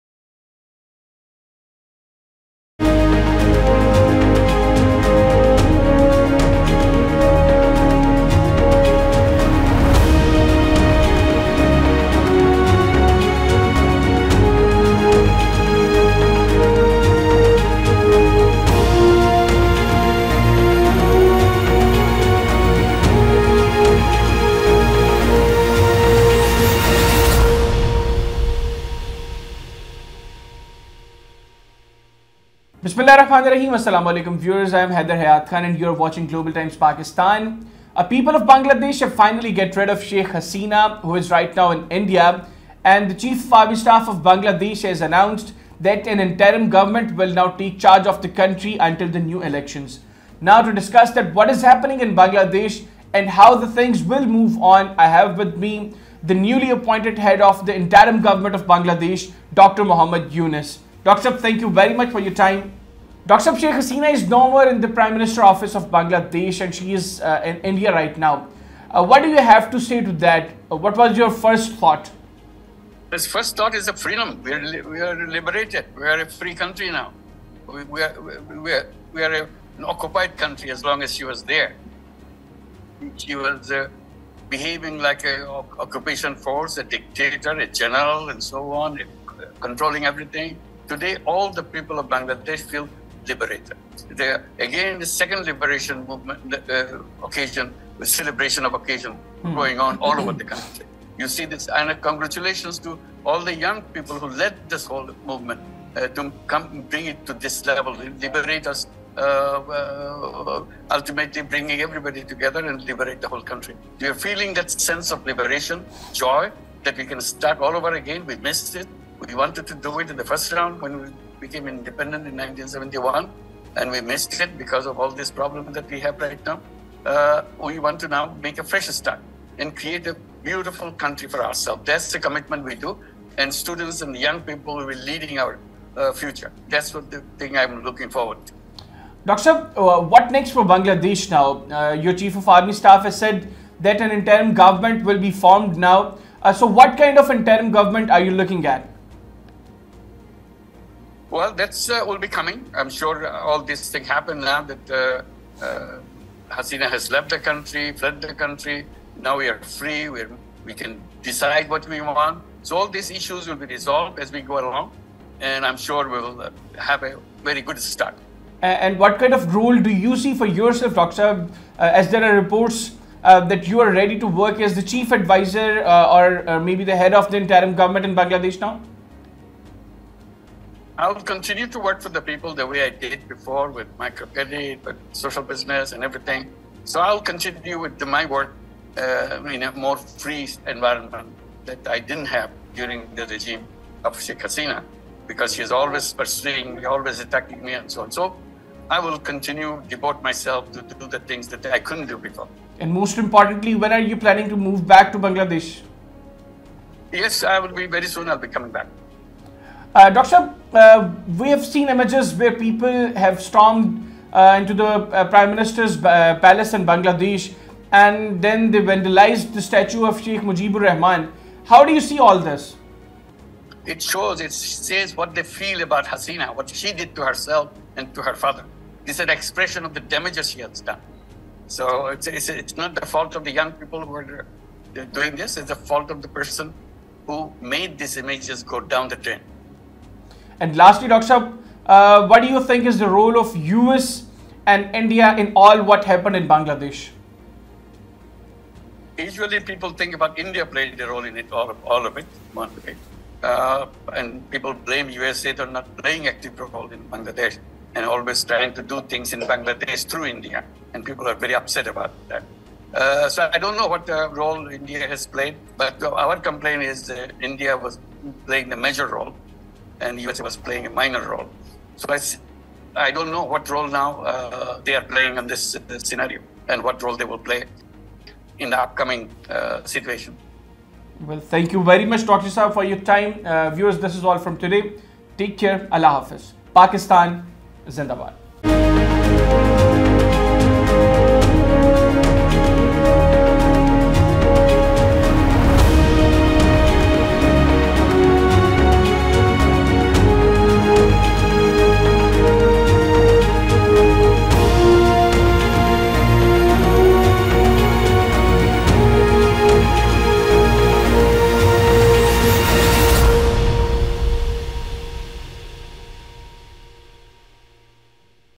Cruel Role Of India In Bangladesh | Dr. Muhammad Yunus Exclusive Interview | Global Times Pakistan
Cruel Role Of India In Bangladesh | Exclusive Interview Of Dr. Muhammad Yunus| Global Times Pakistan